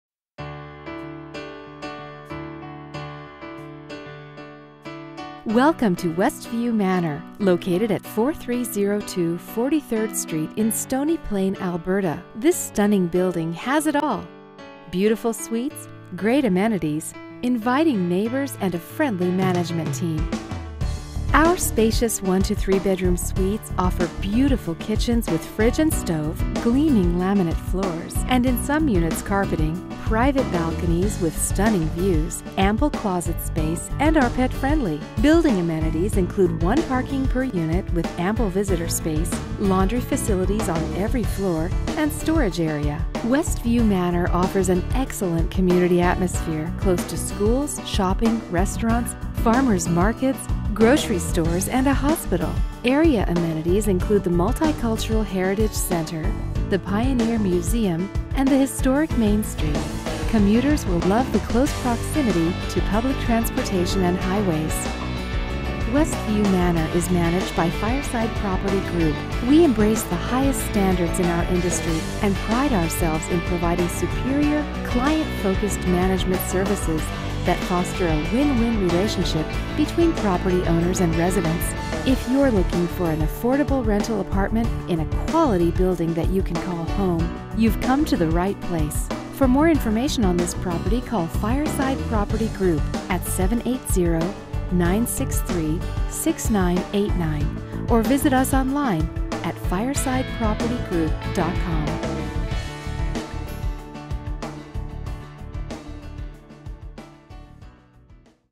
West View Manor- 4302 43rd Street in Stony Plain, Alberta a Fireside Property Group complex
This video was produced by the RentSeeker.ca team for  Fireside Property Group.
 It features a property called West View Manor located at 4302 43rd Street in Stony Plain, Alberta that is managed by  Fireside Property Group.
West View Manor is a townhouse style property that offers beautiful and spacious units and is located near many amenities, transportation and farmer markets. Video by RentSeeker- online apartment rentals made easy !